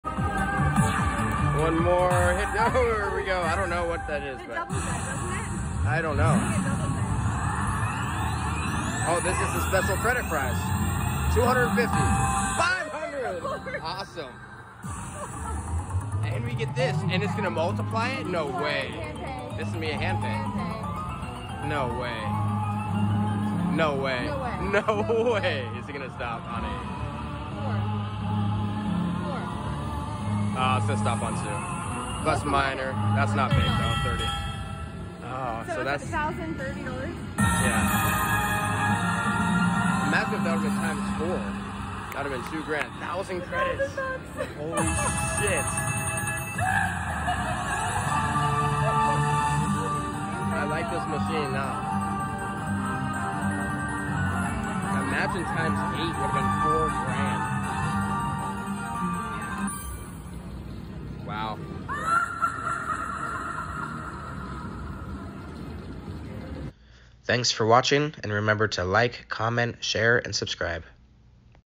⭐️ NICE BIG BONUS 💰FULL SCREEN OF PUMPKINS! 🎃 JACKS WINNING SPELL
First time playing Jacks Winning Spell and got super lucky to get a full screen of pumpkins on our first bonus! 🤑🥳
Max Bet $6/spin at WinStar Casino
Recorded Jan 28th 2021